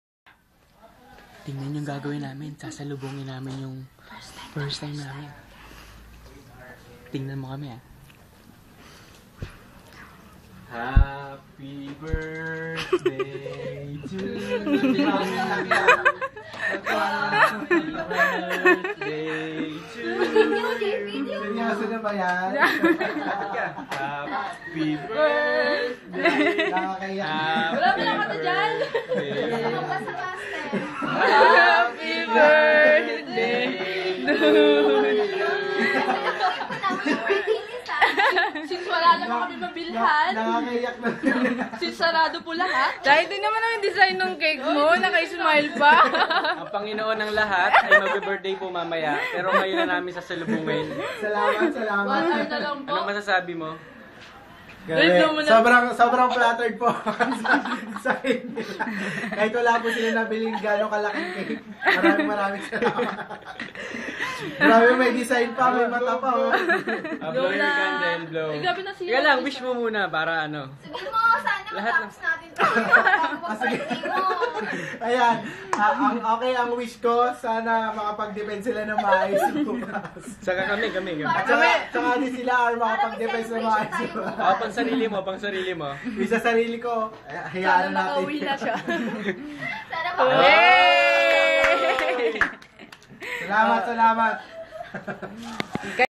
Happy Birthday Cyril Dave Flor!!!
Surprised Celebration! Pagsalubong sa aming kaibigan na si Cyril Dave Flor! Binigyan namin sia ng Cake!